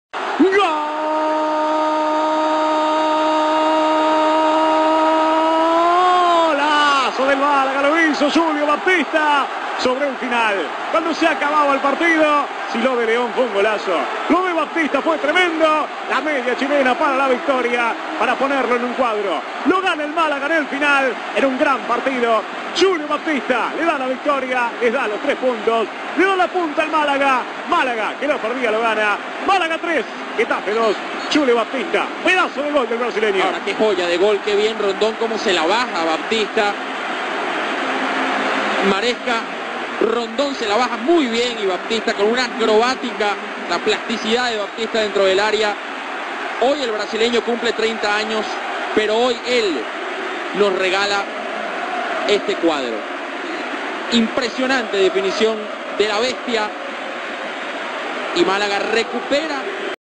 Gol de Julio Baptista mágala x Getafe 3-2
golaço de julio baptista